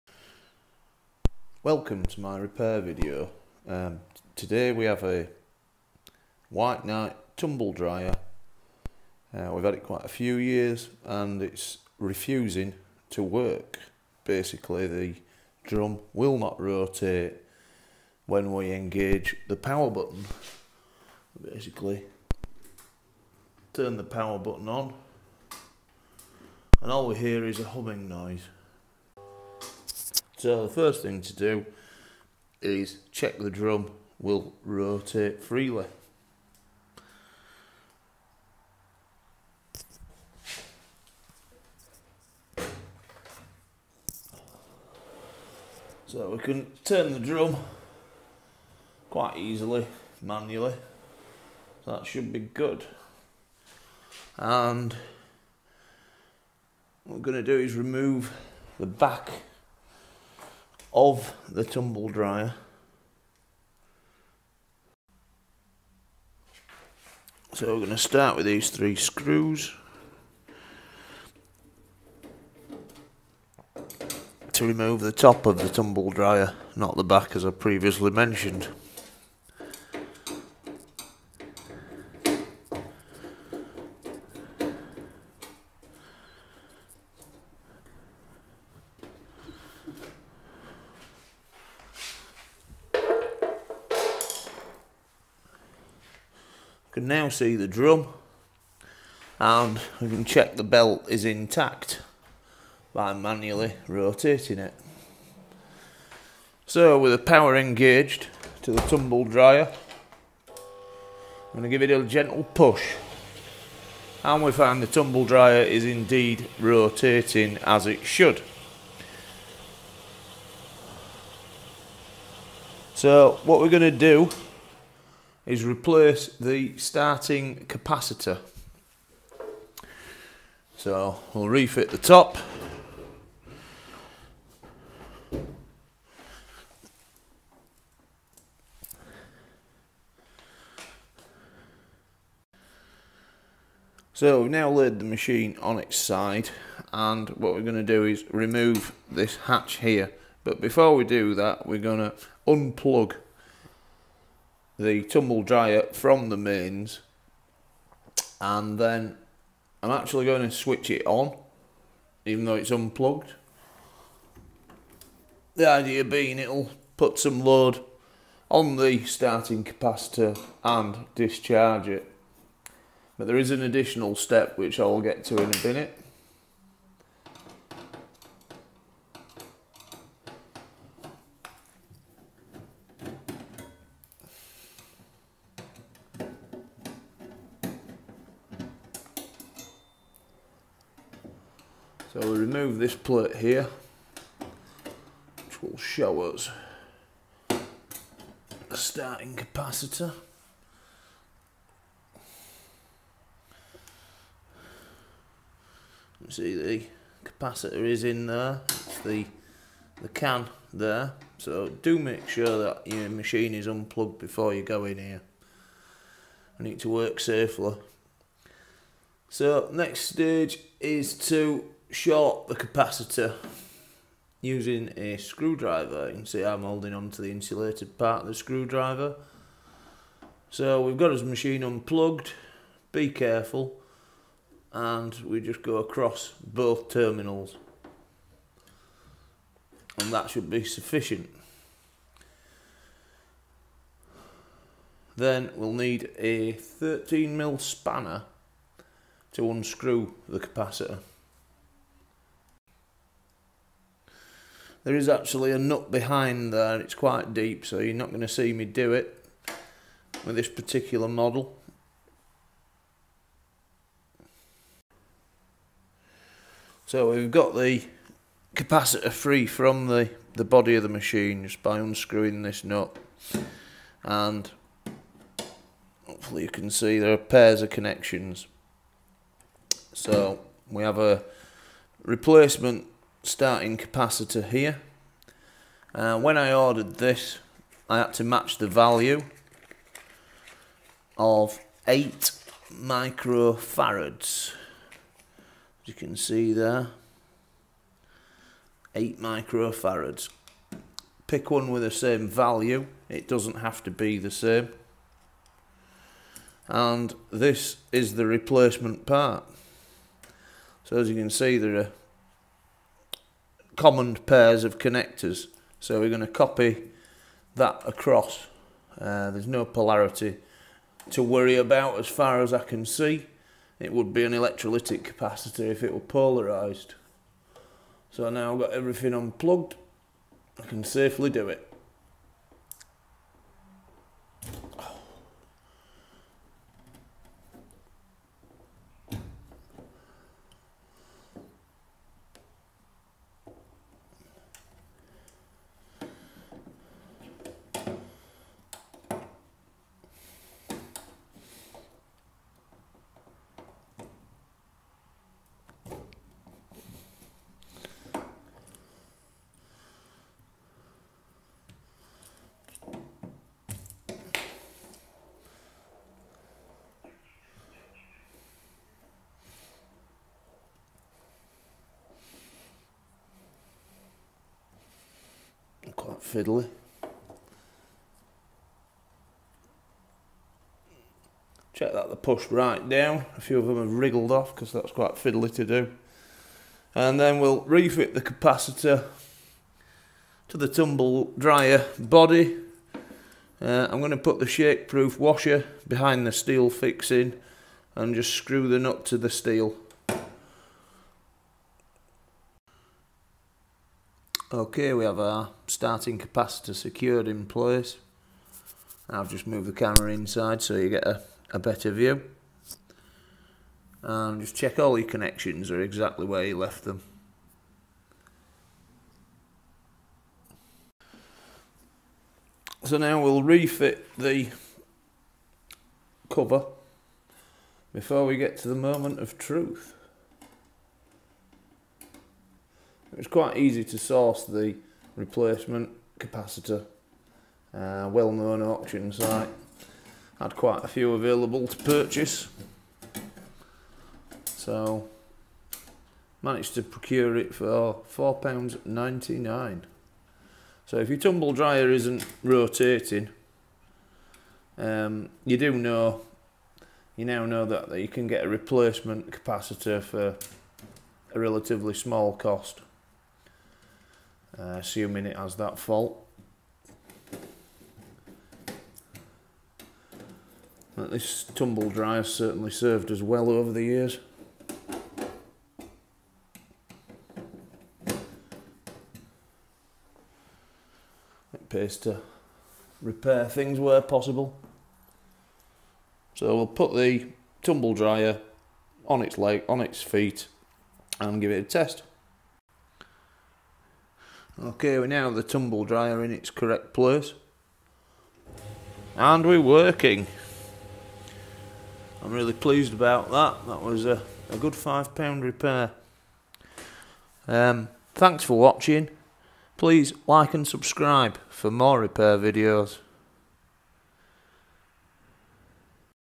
White Knight CL382WV Drum Tumble Dryer not Turning / Rotating Repair.
In this video I repair a White Knight tumble dryer that won’t rotate the drum. If your tumble dryer is make a buzzing noise and not turning the drum the likely cause of the fault is the motor start capacitor. I will show you how to replace this capacitor assuming you have a White Knight CL382WV Tumble dryer. A similar procedure will be required for other models. Work safely with the mains electricity disconnected.

Timestamps:-

0:01  Introduction (the fault).
0:20  The symptoms.
01:05 Removing the top of the tumble dryer.
01:50 Making the dryer drum turn manually.
02:26 Removing the access cover (hatch) from the underside of the tumble dryer.
02:46 Discharging and replacing the old starting capacitor.
07:02 Refitting the access cover to the underside of the tumble dryer.
08:14 Testing the tumble dryer after the repair.